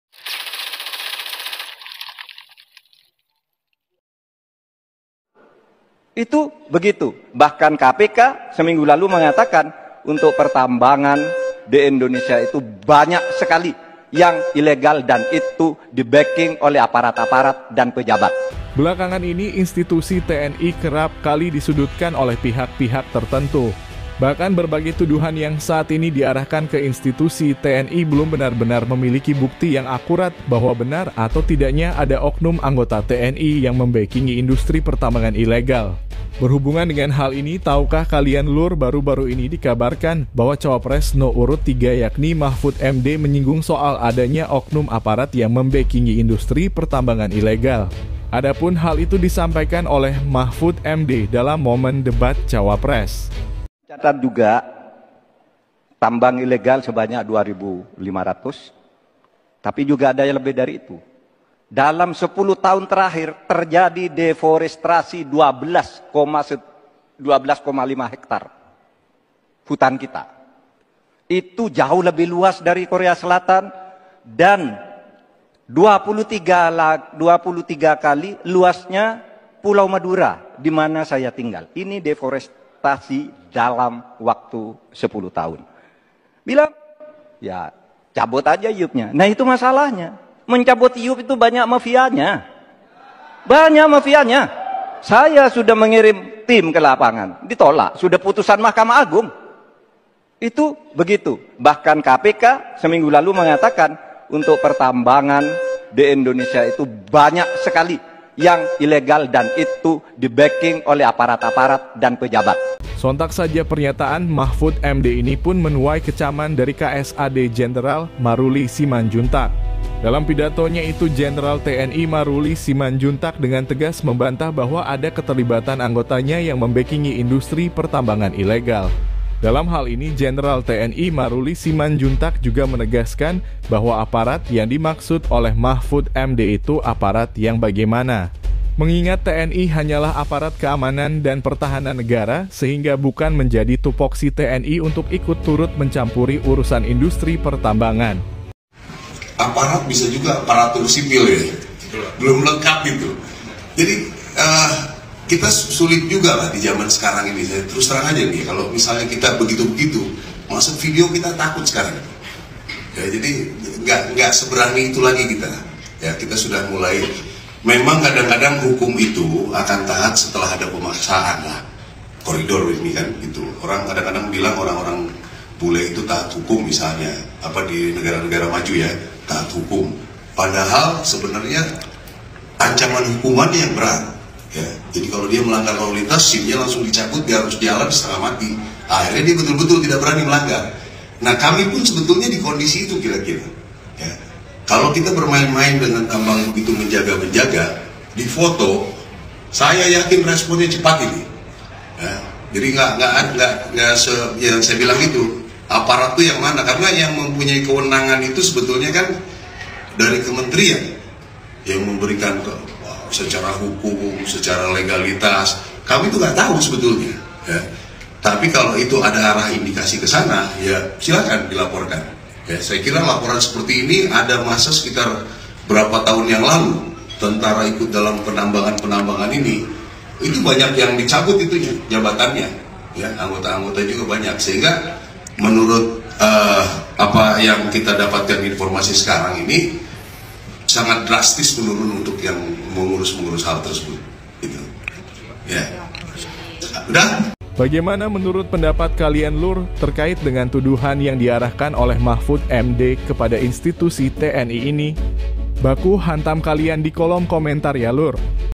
KSAD Maruli Skakmat Mahfud MD, Sok Bicara Data Tuduh TNI Akhirnya Kena Mental Juga
Belakangan ini institusi TNI kerap kali disudutkan oleh pihak pihak tertentu, bahkan berbagai tuduhan yang saat ini di arahkan ke institusi TNI belum benar benar memiliki bukti yang akurat bahwa benar atau tidaknya ada oknum anggota TNI yang membekingi industri pertambangan ilegal. Berhubungan dengan hal ini taukah kalian lur baru baru ini dikabarkan bahwa cawapres no urut 3 yakni Mahfud MD menyinggung soal adanya oknum aparat yang membekingi industri pertambangan ilegal, adapun hal itu disampaikan oleh Mahfud MD dalam momen debat cawapres. Sontak saja pernyataan Mahfud MD ini pun menuai kecaman dari KSAD Jenderal Maruli Simanjuntak. Dalam pidatonya itu Jenderal TNI Maruli Simanjuntak dengan tegas membantah bahwa ada keterlibatan anggotanya yang membekingi industri pertambangan ilegal, dalam hal ini Jenderal TNI Maruli Simanjuntak juga menegaskan bahwa aparat yang dimaksud oleh Mahfud MD itu aparat yang bagaimana?, mengingat TNI hanyalah aparat keamanan dan pertahanan negara, sehingga bukan menjadi tupoksi TNI untuk ikut turut mencampuri urusan industri pertambangan.